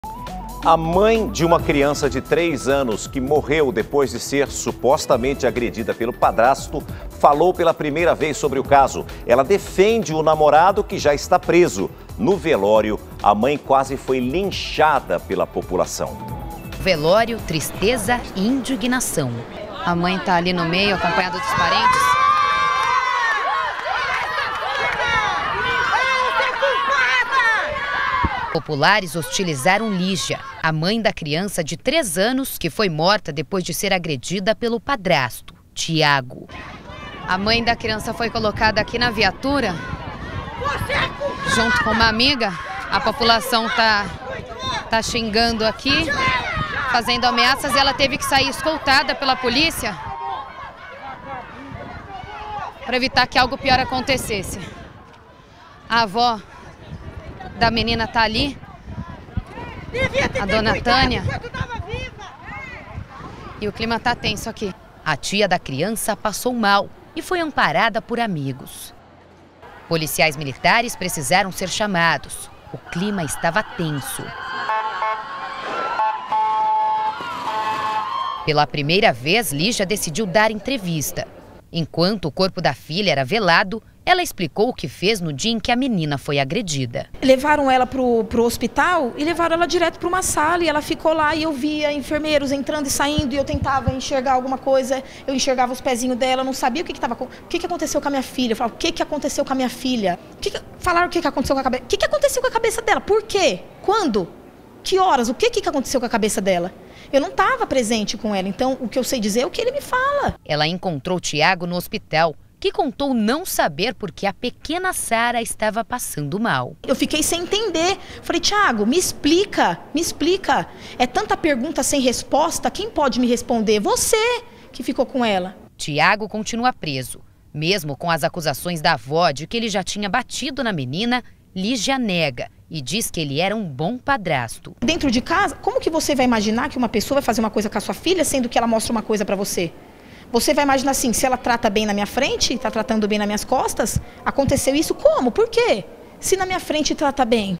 Mãe defende namorado que matou filha de 3 anos
A mãe da criança de 3 anos, que morreu depois de ser supostamente agredida pelo padrasto, defendeu o namorado das acusações. Durante o velório, populares hostilizaram a mulher, que quase foi linchada e precisou de escolta policial. Veja!